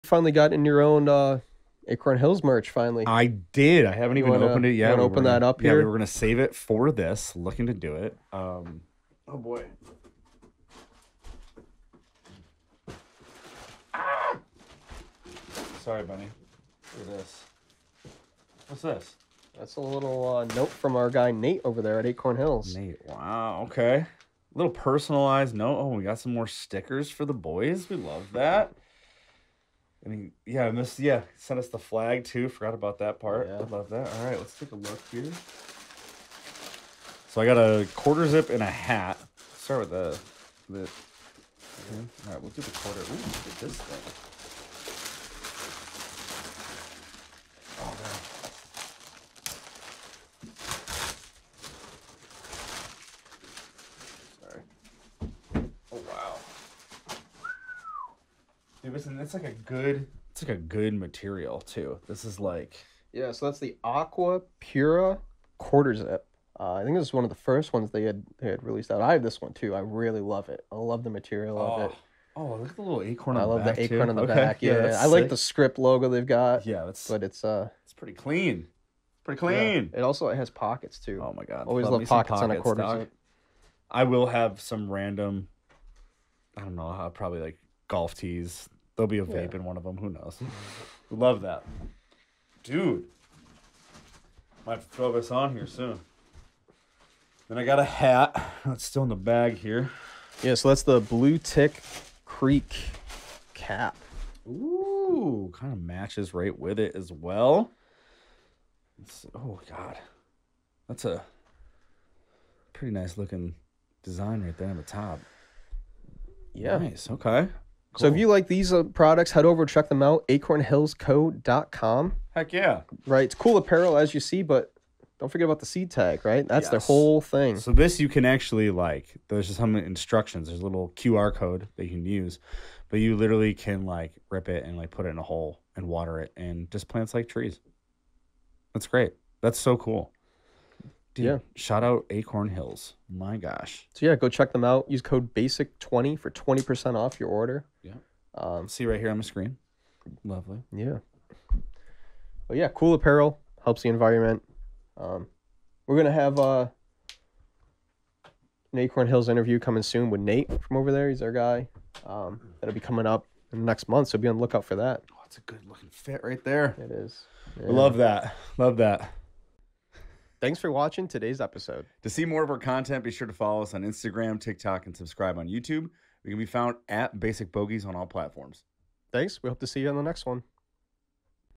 Opening the Acorn Hills Care Package!
Gary opens up his first Acorn Hills care package. Be sure to go check them out at acornhillsco.com